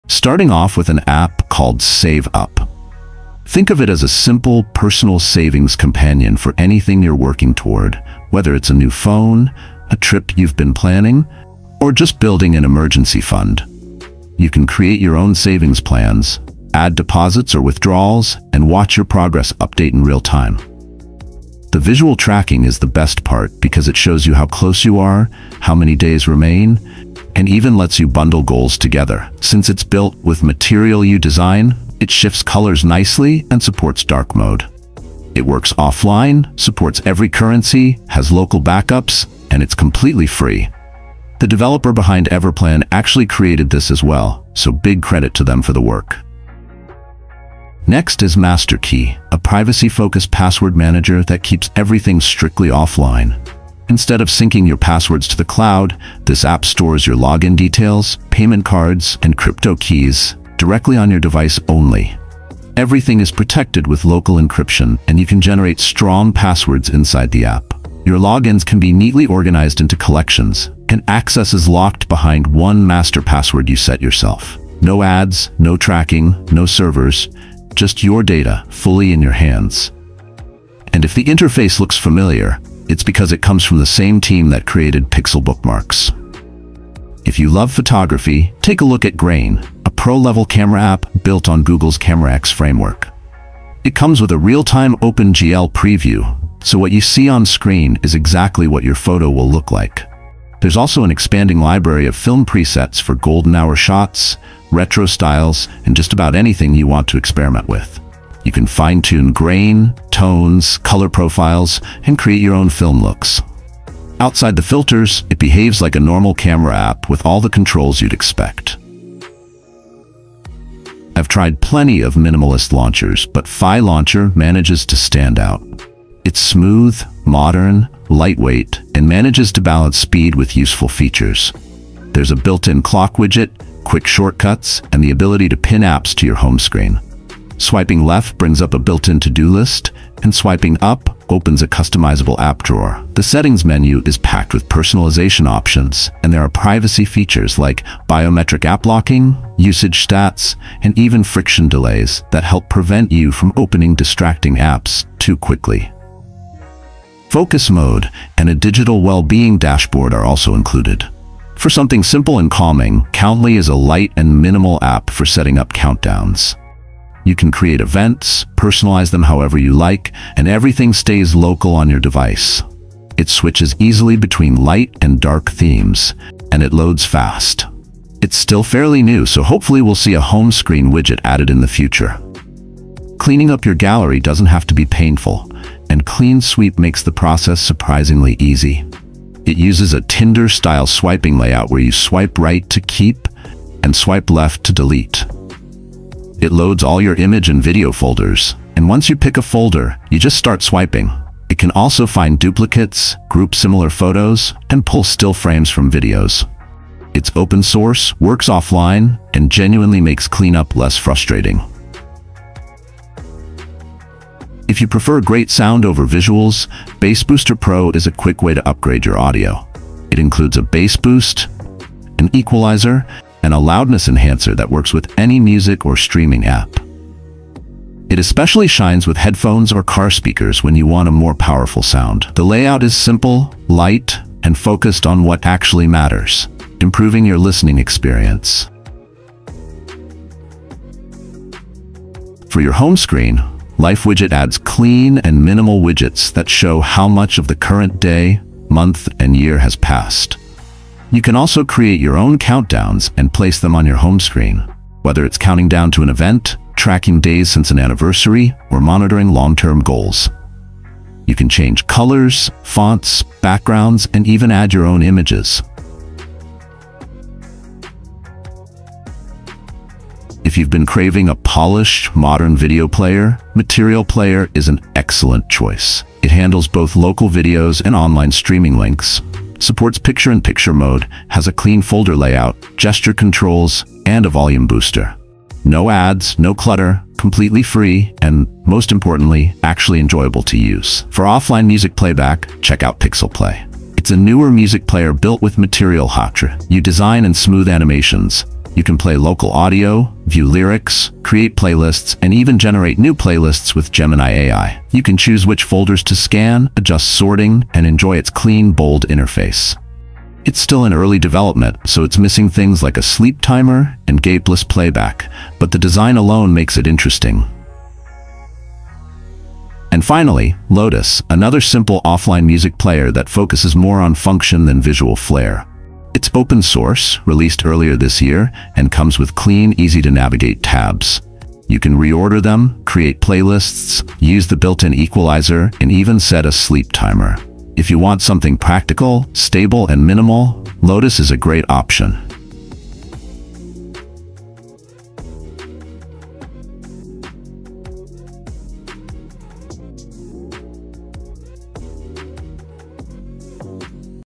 11 Underrated Android Apps That Quietly Upgrade Your Phone
In this video, we explore 11 underrated Android applications that can instantly improve how your phone feels and functions. These apps focus on productivity, privacy, clean design, and smarter everyday use. If you enjoy simple, useful tools that actually make a difference, you’ll find some real gems here.

Apps featured in this video:

Save Up, a simple savings tracker for planning purchases and goals,

Master Key, an offline-first password manager with full encryption,

Grain, a powerful camera app with realtime film-style previews,

Clean Sweep, a swipe-based gallery cleaner that removes clutter fast,

Bass Booster, an audio enhancer that improves sound quality,

Life Widget, minimalist home-screen widgets for time and progress tracking,

Material Player, a modern video player for local and online playback,

Lotus, a practical and open-source music player with equalizer and sleep timer.

If you know other new or underrated Android apps worth checking out, share them in the comments — I might include them in part two.